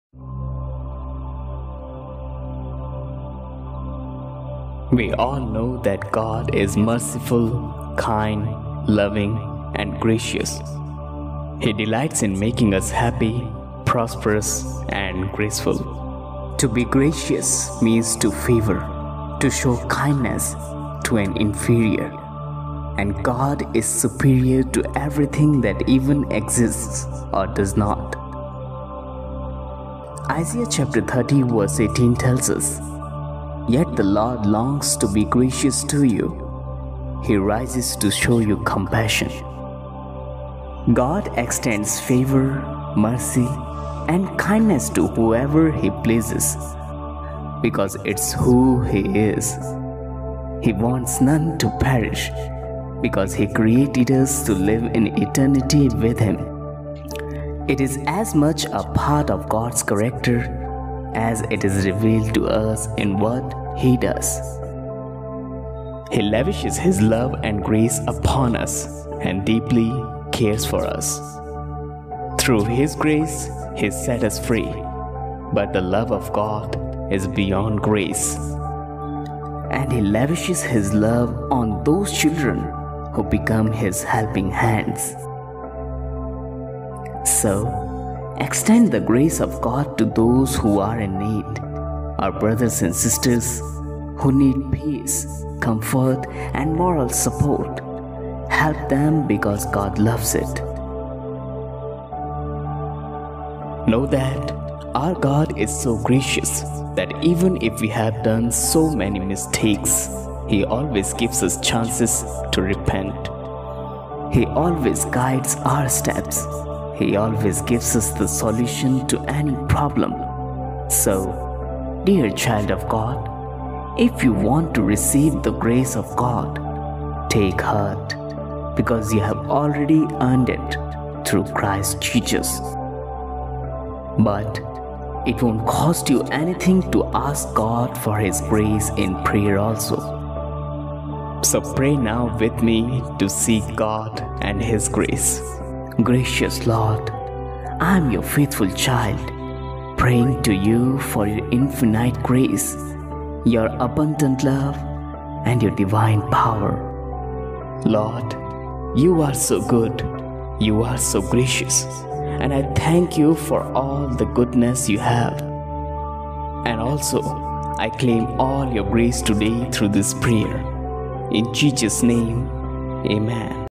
"YOUR EYES ARE BLESSED THAT YOU ARE SEEING THIS RIGHT NOW" - GOD | Hear It Now👆| Lord Helps Ep~333
Brief Introduction Of This Video 👇👇

"We All Know That God Is Merciful, Kind, Loving, And Gracious.
He Delights In Making Us Happy.

God Extends Favor, Mercy, And Kindness To Whoever He Pleases Because It’s Who He Is. He Wants None To Perish Because He Created Us To Live In Eternity With Him. It Is As Much A Part Of God’s Character As It Is Revealed To Us In What He Does. He Lavishes Love And Grace Upon Us And Cares Deeply For Us. Though Grace Set Us Free, But The Love Of God Is Beyond Grace.

But It Won’t Cost You Anything To Ask God For His Grace In Prayer Also.
So Pray Now With Me To Seek God And His Grace –

Music Credit -

1》 This video is made for Teaching/Motivational purposes.
2》This video has no negative impact on anyone. (Inspired by Joel Osteen)

Team,
Lord Helps